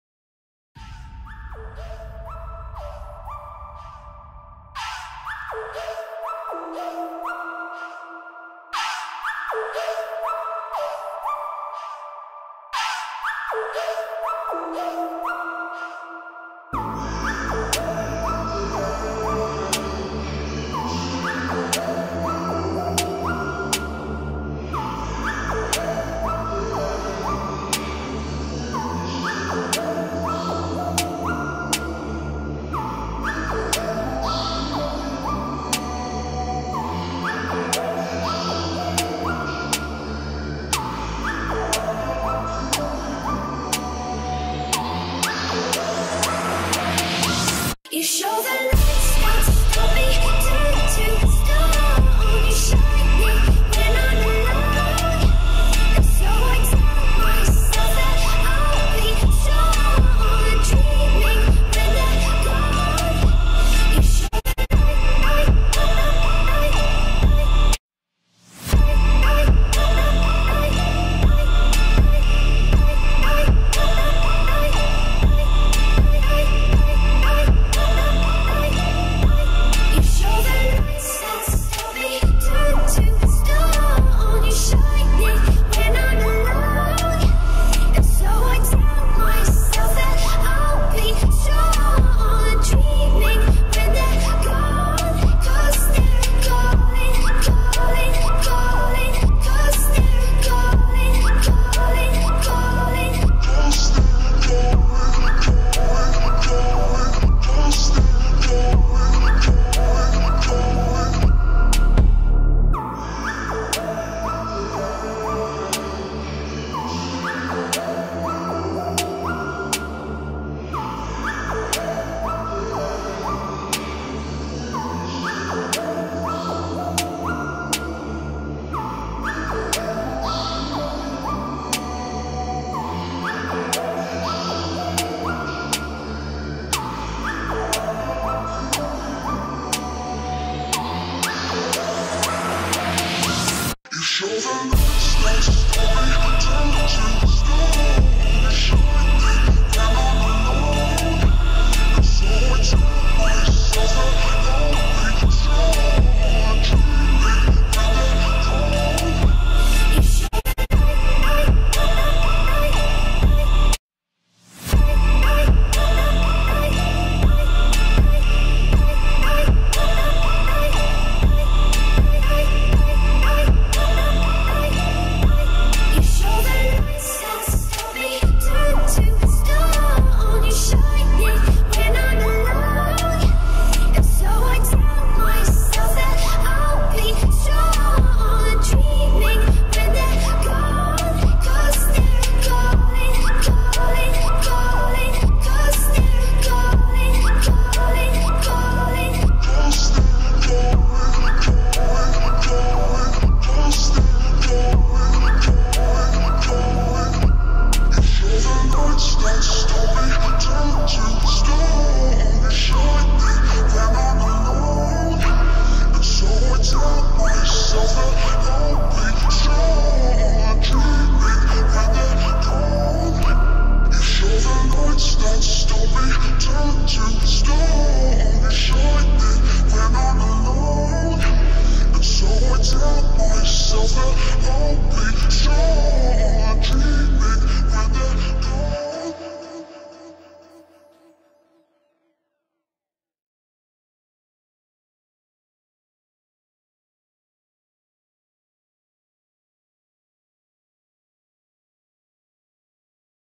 Solis - Speed Paint
Drew Allookazoo's oc for my part of the ar trade :)